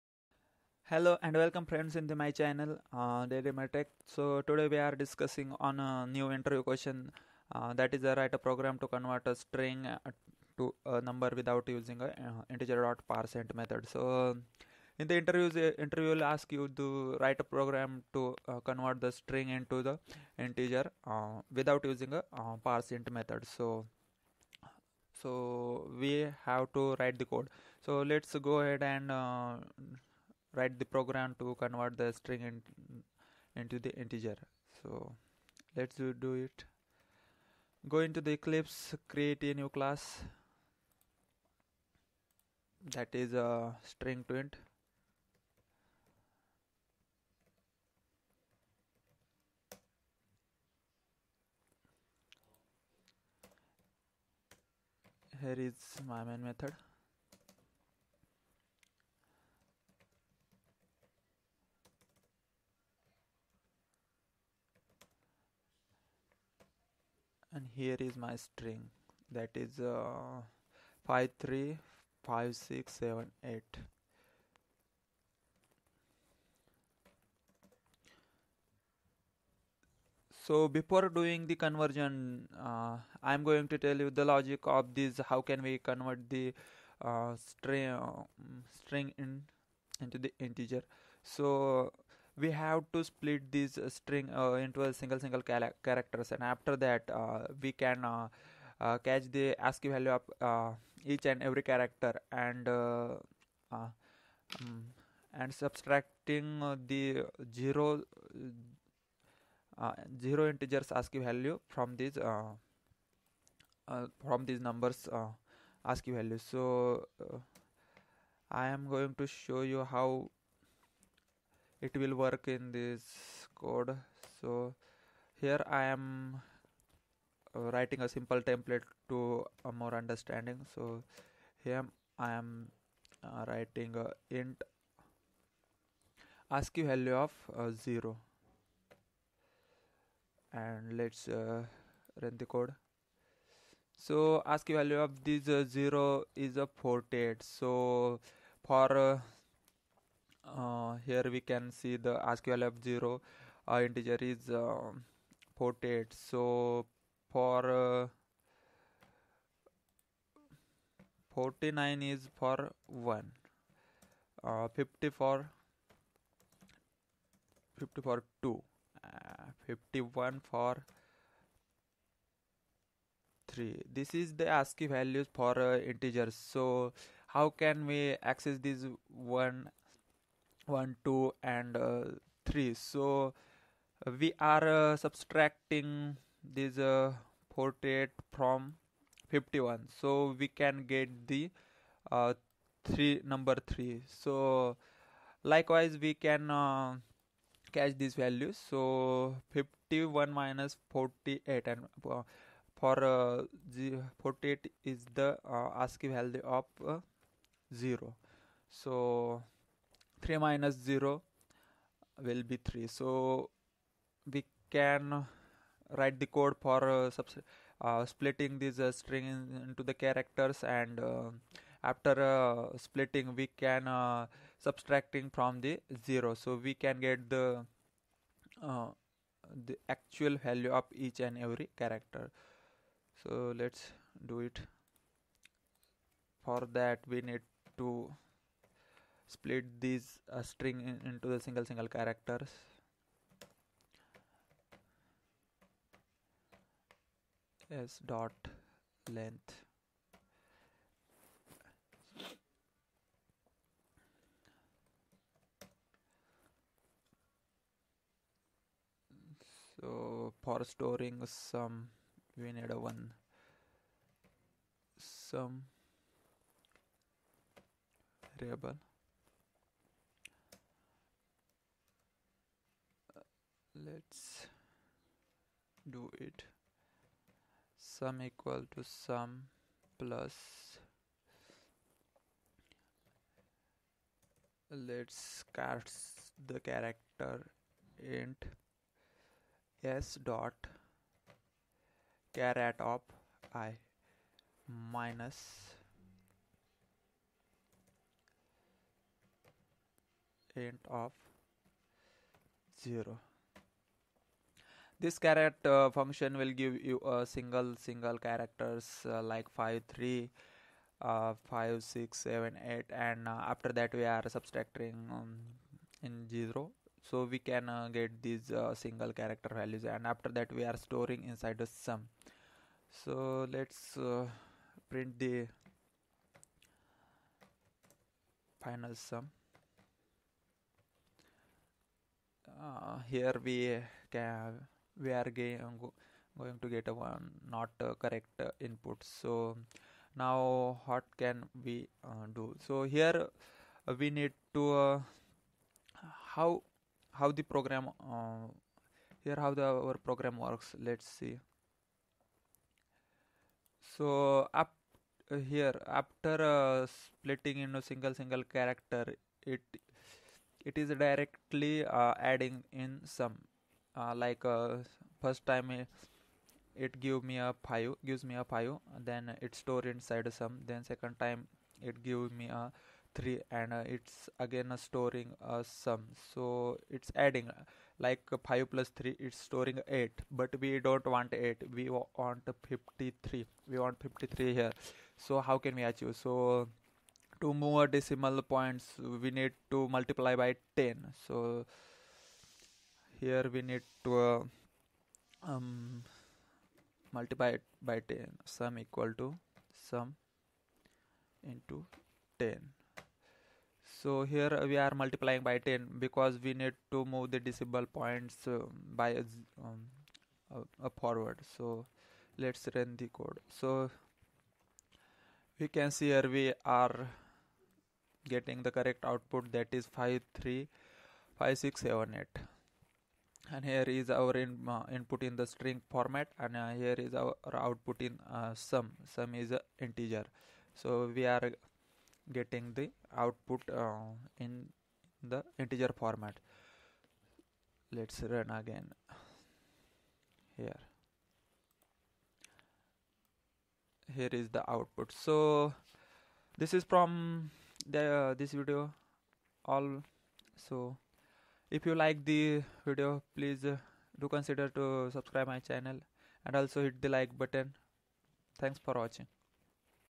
convert string into integer without using Integer.parseInt() method in java and valueOf
how to convert string value into integer value without using Integer.parseInt() method in java and valueOf